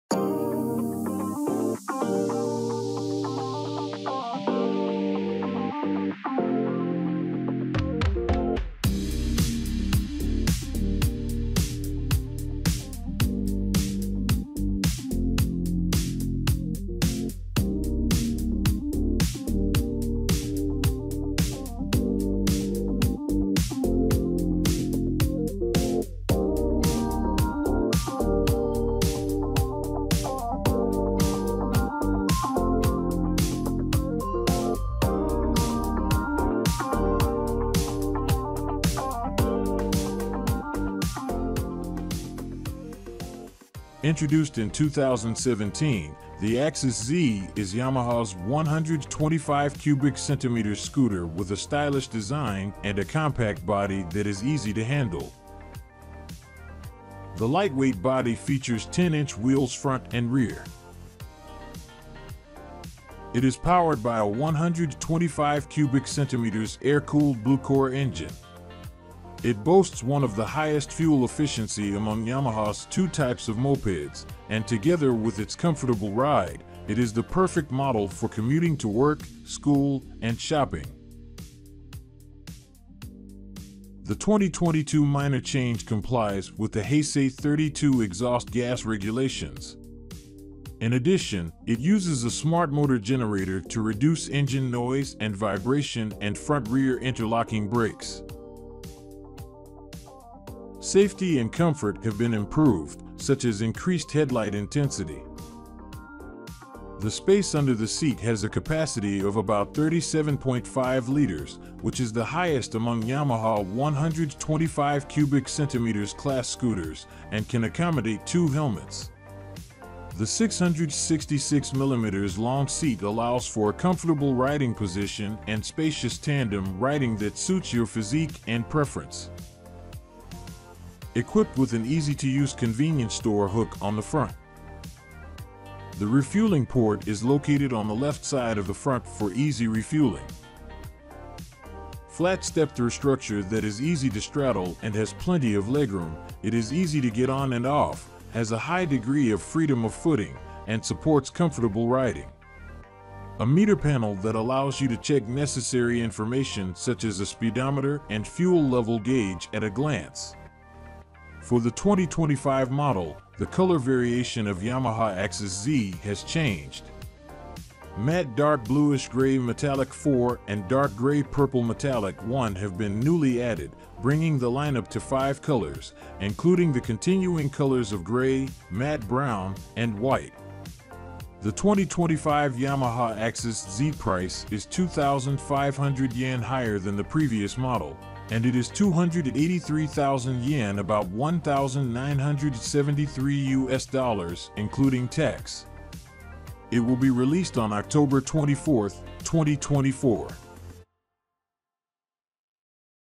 2025 Yamaha Axis Z: Sleek New Colors and Enhanced Features | Official Release Date and Specs
The Yamaha Axis Z is a 125cc scooter with a stylish design and a compact body. It is powered by a 125cc air-cooled blue-core engine and offers excellent fuel efficiency and a comfortable ride. The 2025 model features a new color variation and a slightly increased price of 283,000 yen. It will be released on October 24, 2024.